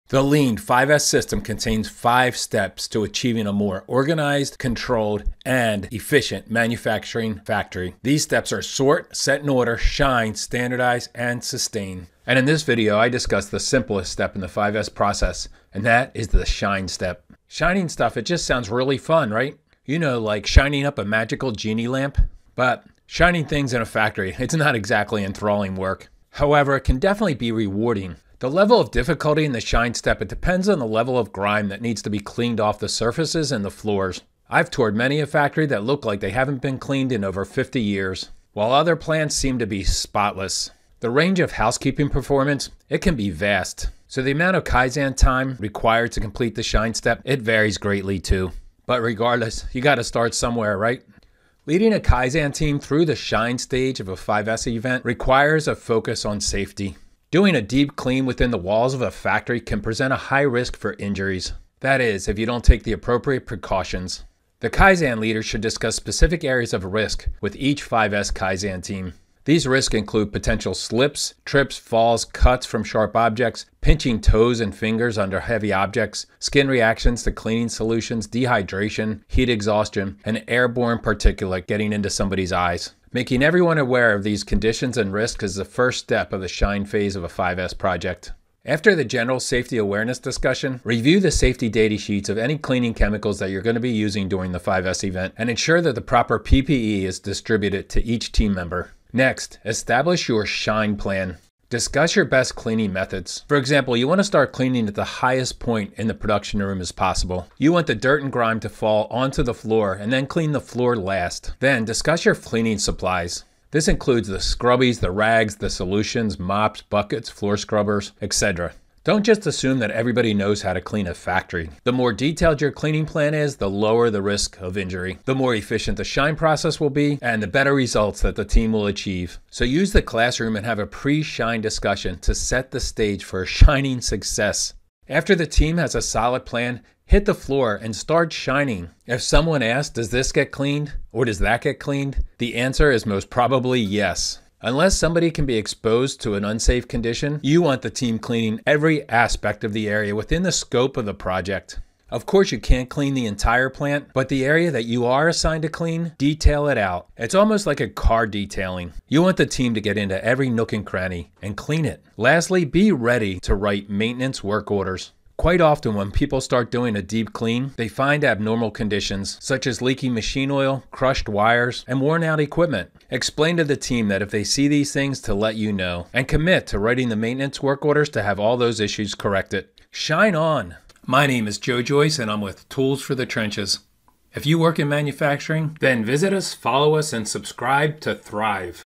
Lean 5S - SHINE
This is video #4 out of 6 in this series about Lean 5S. This is a great series to use as part of your kaizen event training.

The lean 5S system contains 5 steps to achieving a more organized, controlled and efficient manufacturing plant. These steps are Sort, Set-in-Order, Shine, Standardize and Sustain. And in this video, I discuss the simplest step in the 5S process, and that is the Shine step. Shinning things in a factory isn’t exactly enthralling word but it can definitely be rewarding.

Leading a kaizen team through the Shine stage of a 5S event, requires a focus on safety. Doing a deep clean within the walls of a factory can present a high-risk for injuries. That is, if you don’t take the appropriate precautions.

This video is an excellent resource to be used as helpful Kaizen training.